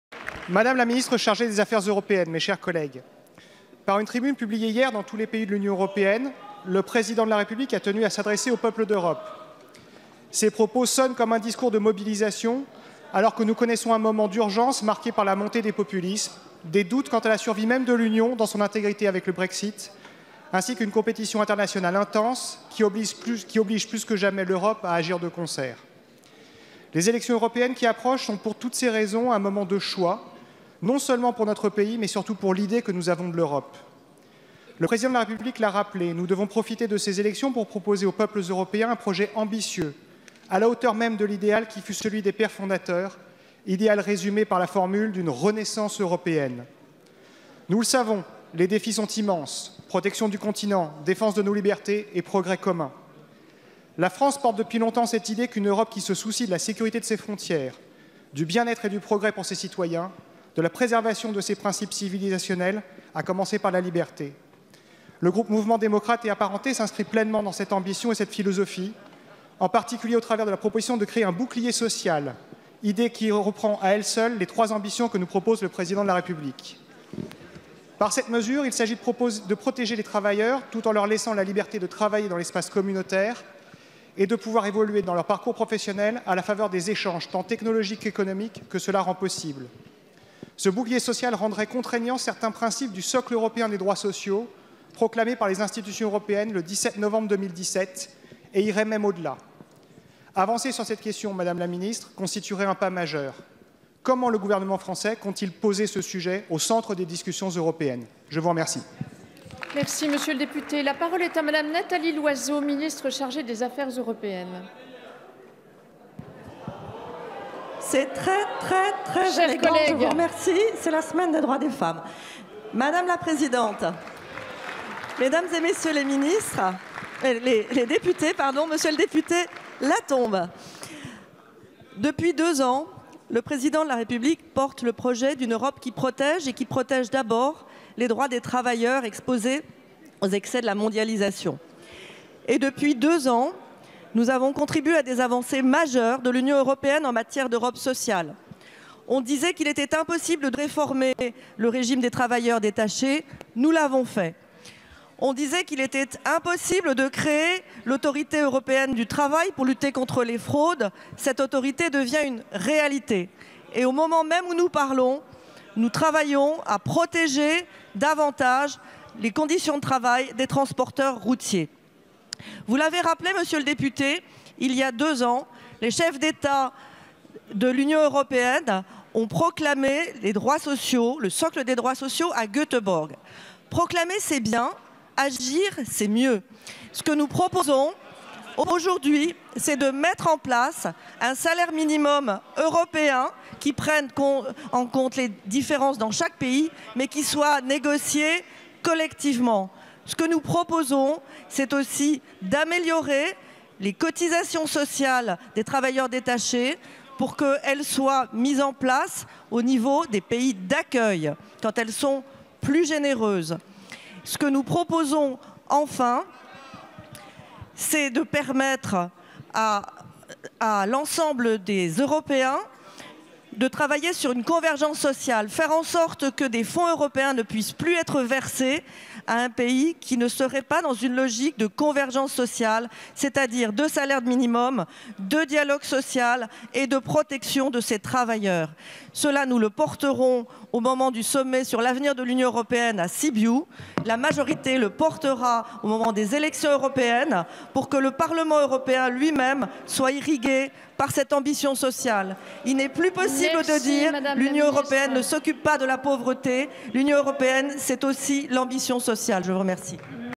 QAG relative à un bouclier social européen - Philippe Latombe
Question au Gouvernement de Philippe Latombe, député de Vendée, relative au bouclier social européen, proposé par le Président de la République Emmanuel Macron dans sa tribune européenne.